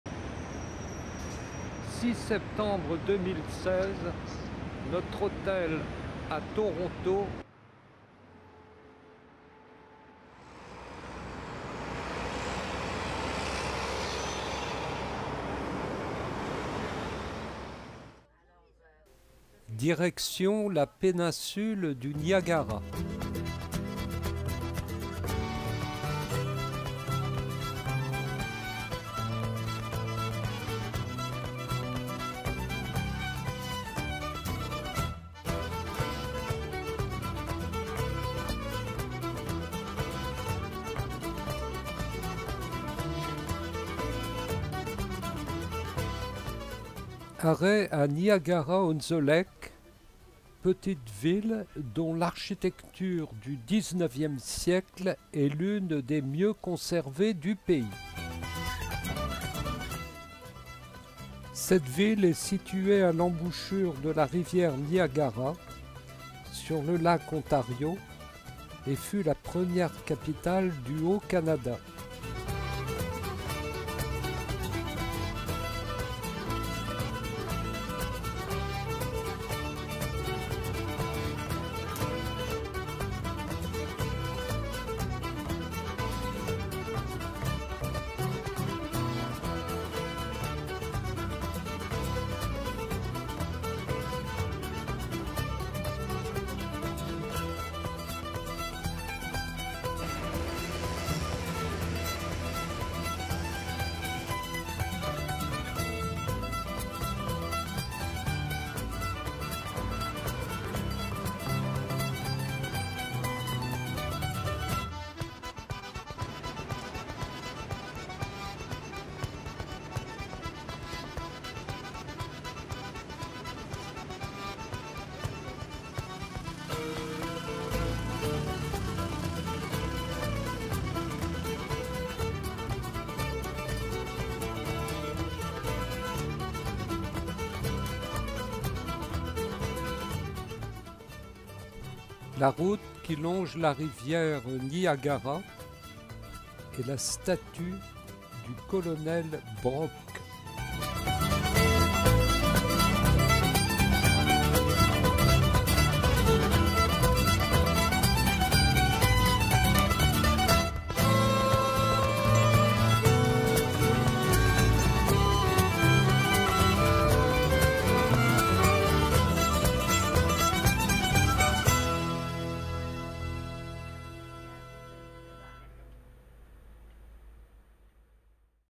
NIAGARA ON THE LAKE ( ONTARIO )
Charmante petite ville sur la route des chutes, en longeant la rivière Niagara.